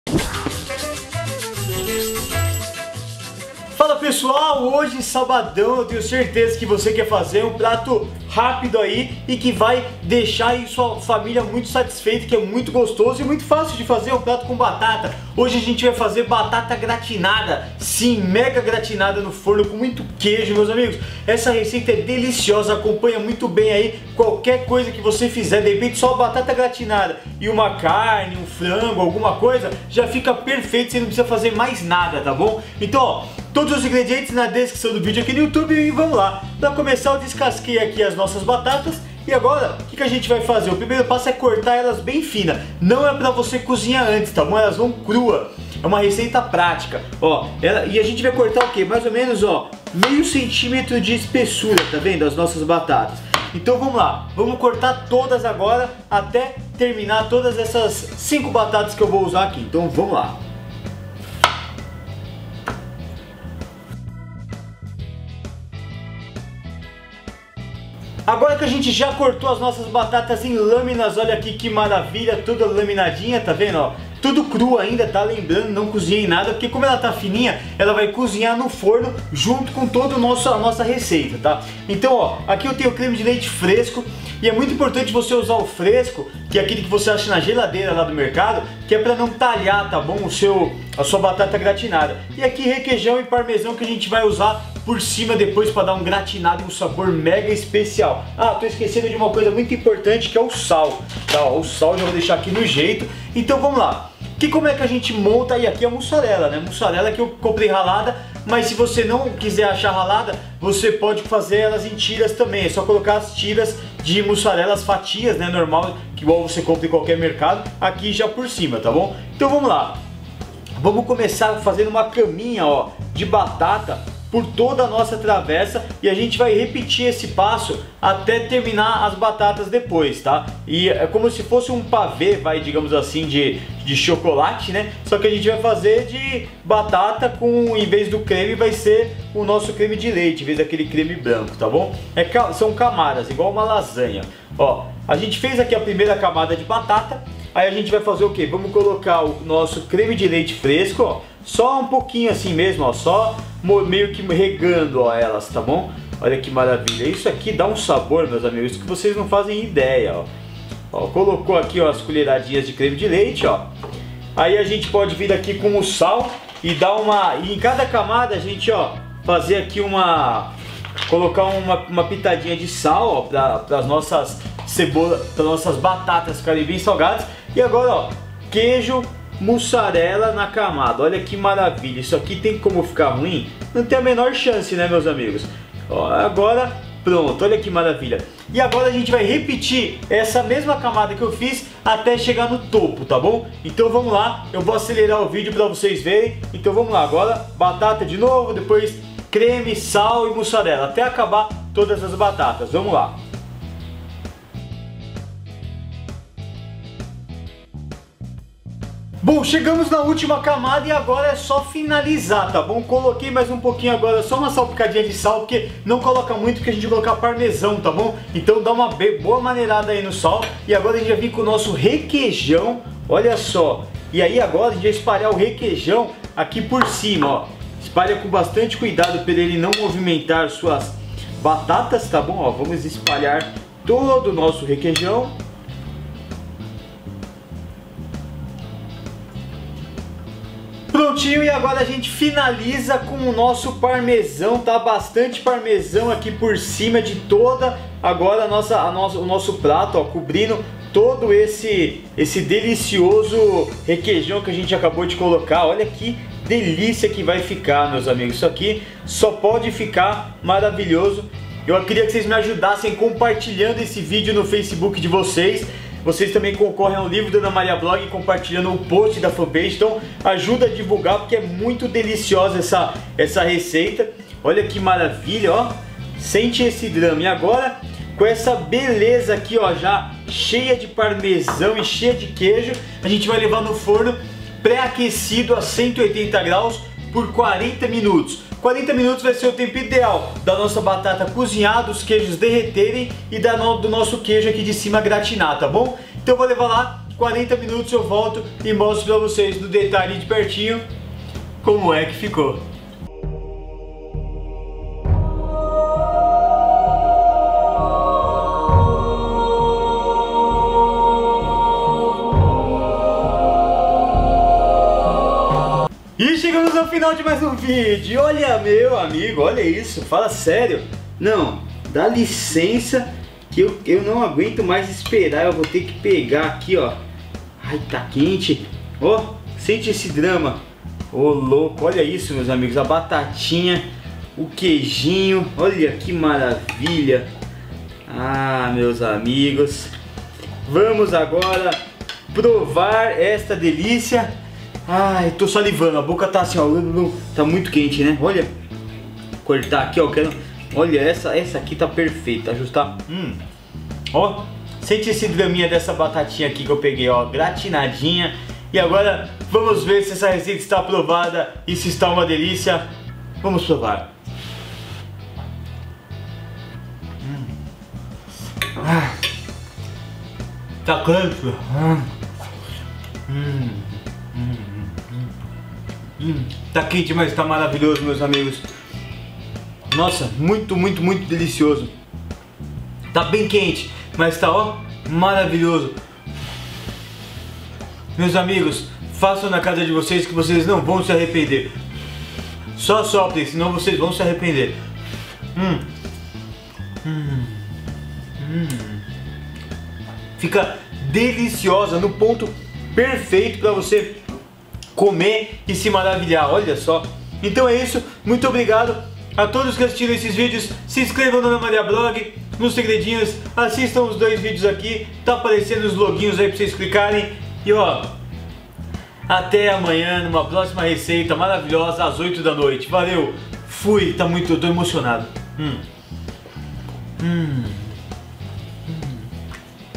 COMO FAZER GRATINADO DE BATATA E QUEIJO
Hoje você vai aprender como fazer um delicioso Gratinado de Batata e Queijo, uma receita muito boa que você poderá fazer tanto para um almoço, quanto para o jantar! Fica uma delícia!

Ingredientes:

- 5 batatas grandes
- 1 garrafinha de creme de leite fresco
- 100g de queijo parmesão ralado
- 1 pote de requeijão
- 200g de mussarela ralada

Modo de preparo:

Assista o vídeo :D

Abraços
Caio Novaes (Brogui)